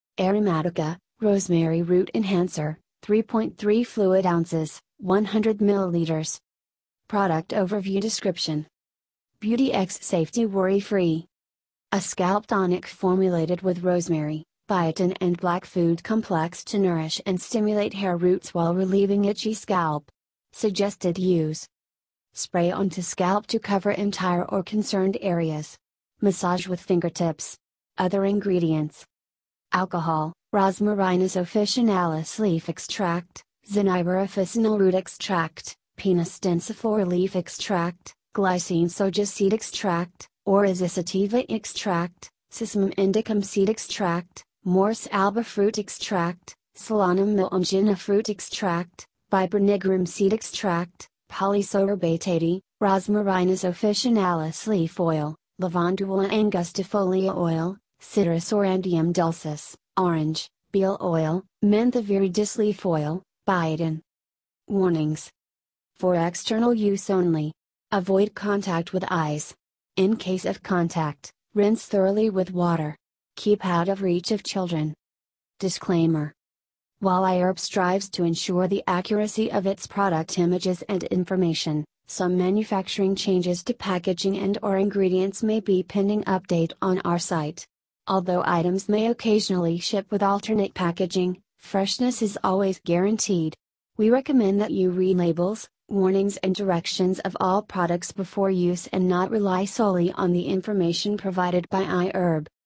Aromatica, Rosemary Root Enhancer, 3.3 fl oz (100 ml)
Product Overview
Description

    Beauty x Safety
    Worry Free

A scalp tonic formulated with Rosemary, Biotin and Black Food Complex to nourish and stimulate hair roots while relieving itchy scalp.
Suggested Use

Spray onto scalp to cover entire or concerned areas. Massage with fingertips.
Other Ingredients

Warnings

For external use only. Avoid contact with eyes. In case of contact, rinse thoroughly with water. Keep out of reach of children.